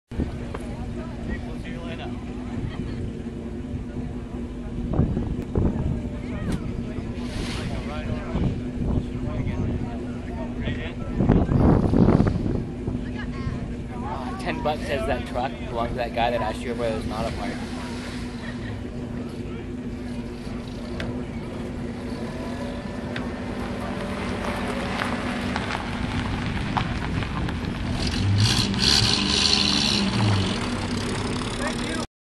Truck leaving Mosman Park
Searsport Maine Mosman Park, Fling into Fall car show. Oct 9th, 2010